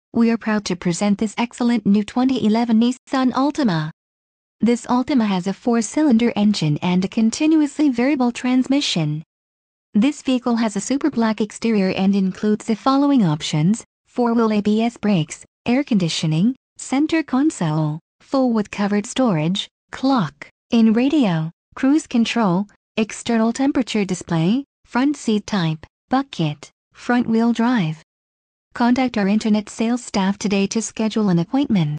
2011 Nissan Altima Executive Jeep Nissan
Executive Jeep Nissan

2011 Nissan Altima 2.5 S
Engine: 2.5L 4 cyls
Trans: Continuously Variable (CVT)
Color: Super Black